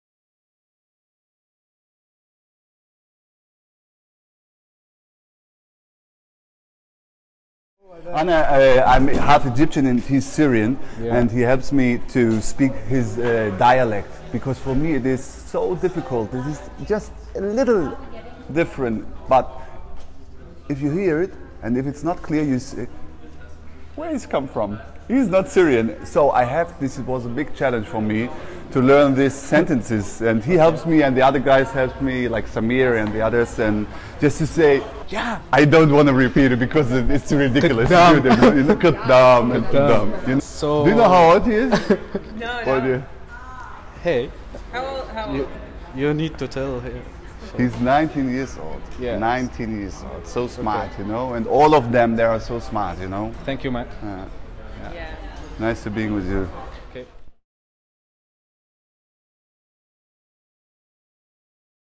No Monsters In Berlin_film_Samir Fuchs_On his Syrian accent
Samir Fuchs (Muazz) chats from the set on how our Syrian cast and crew helped him perfect the Syrian accent.